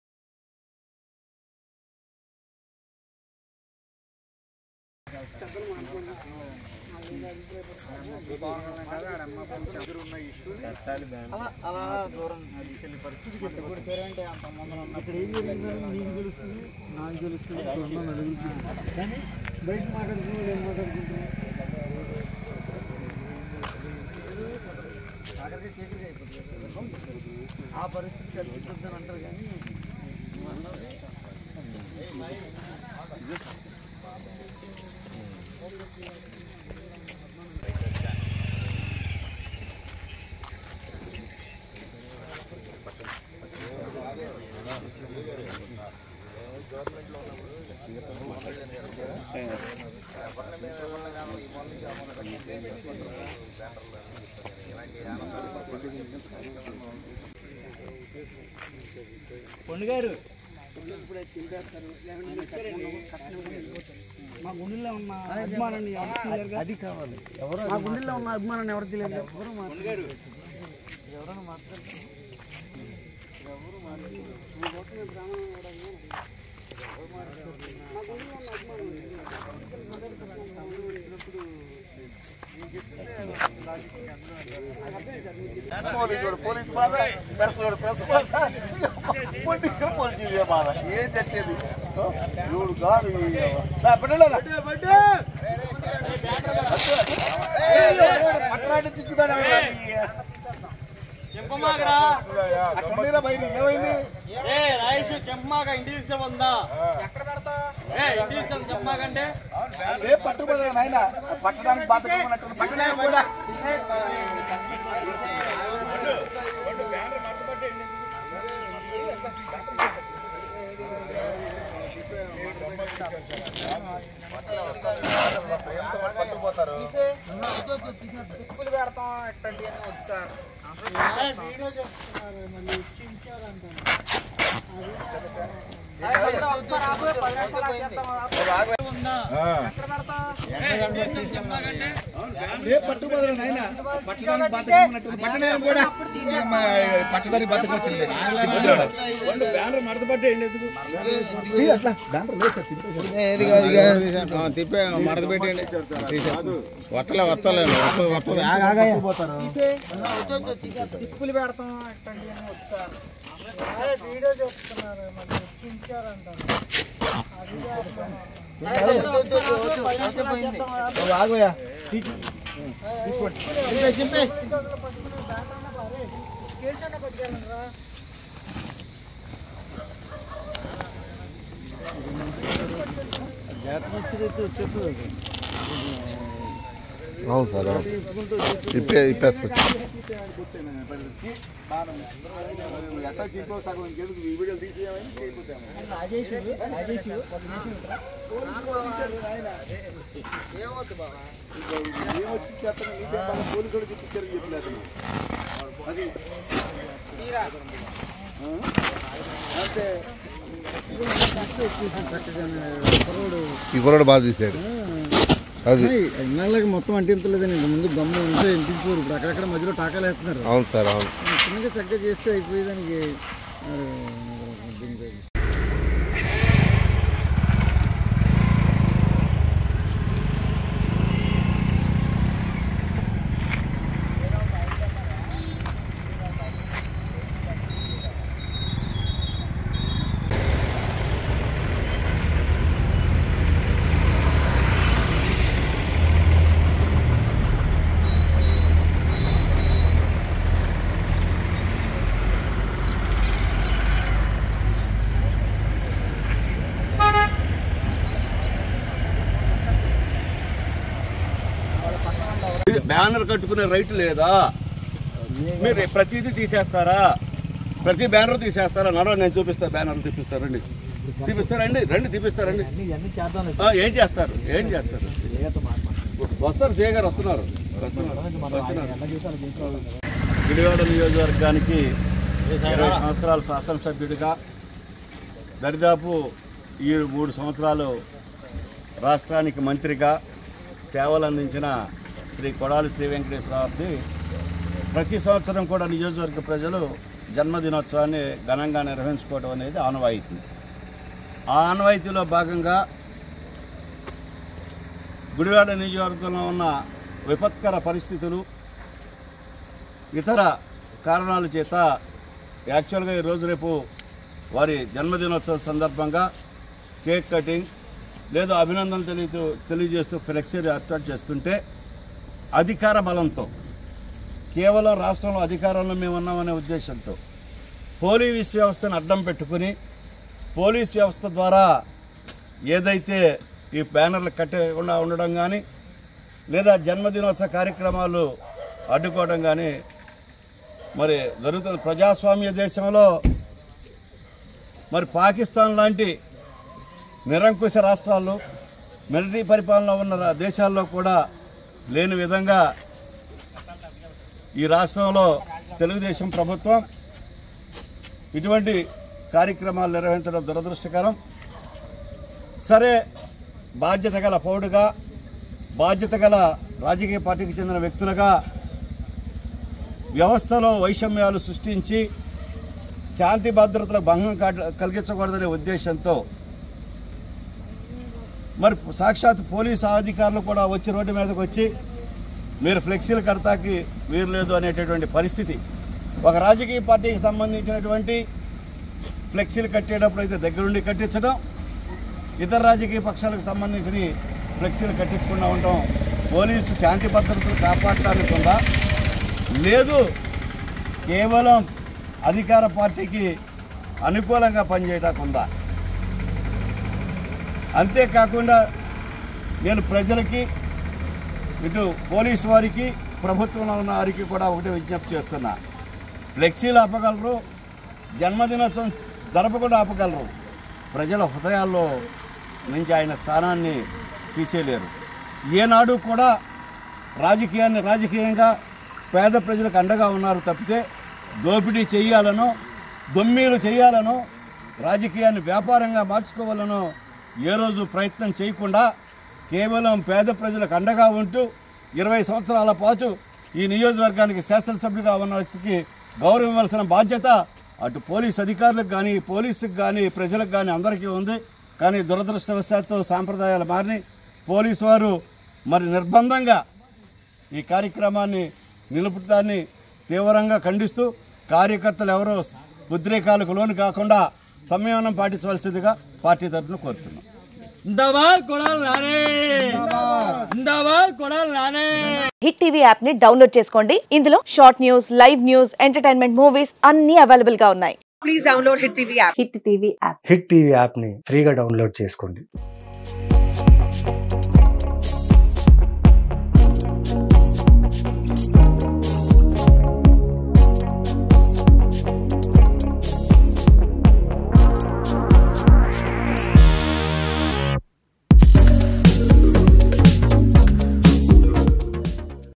గుడివాడలో ఉద్రిక్తత.. || Big Shock to Kodali Nani On His Birthday
Watch : గుడివాడలో ఉద్రిక్తత.. || Big Shock to Kodali Nani On His Birthday @Hittvtelugunews